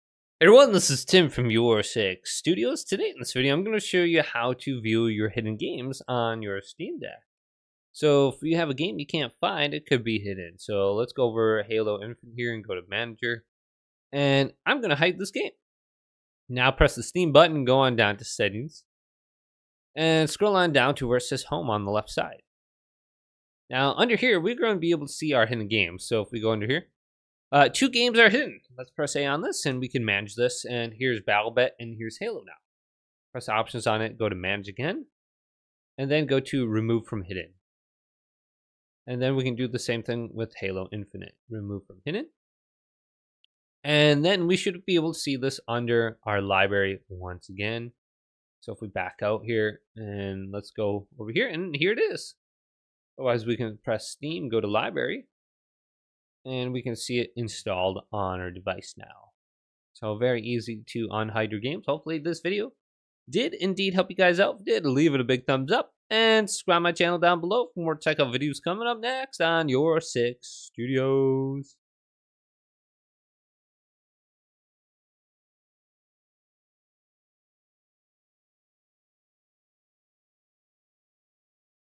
How to view your Hidden Games on your Steam Deck Account (Easy Method)
To view your hidden games on your Steam Deck, follow these steps:

Open Steam: Launch the Steam app on your Steam Deck and log in to your account.

Access the View Menu: In the top left corner, click on the “View” option.
Select Hidden Games: From the dropdown menu, select “Hidden Games.” This will display a list of all the games you’ve hidden from your library